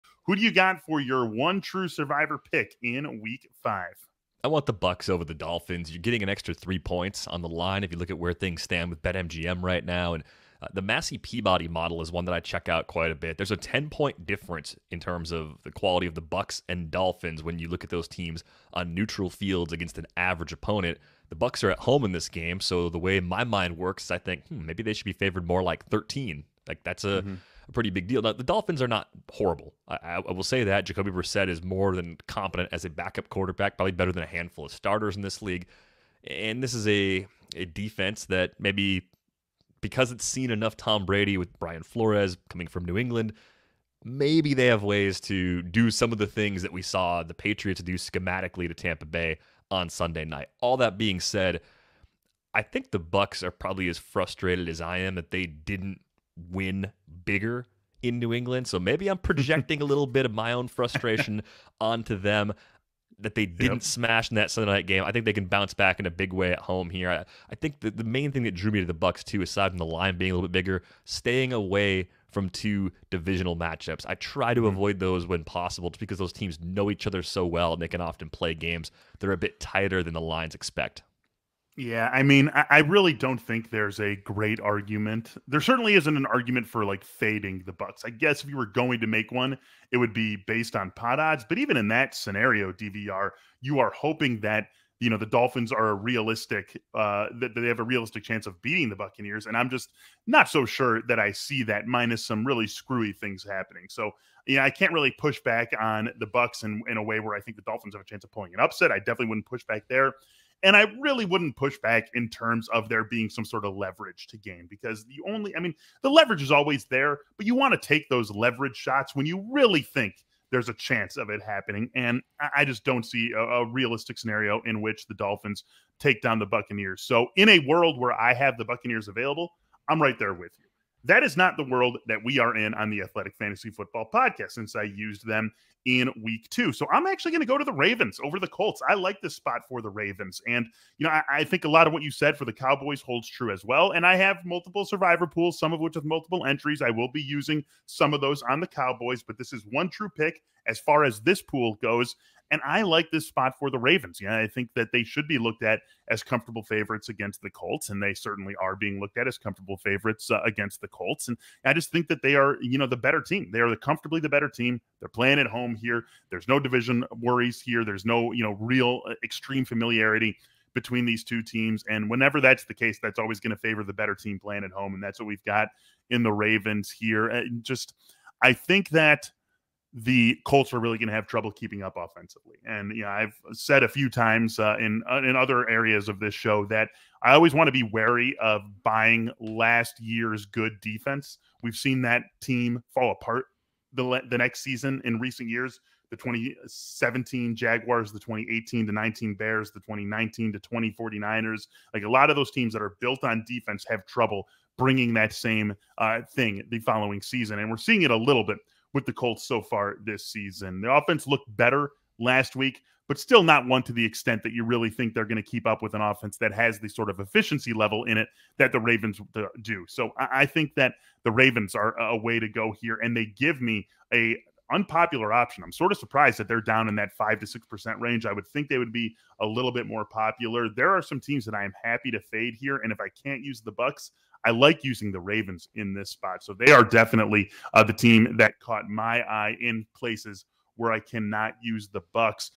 Week 5 NFL Survivor Picks
The Athletic's Michael Beller and Derek VanRiper make their Survivor Pool picks for Week 5.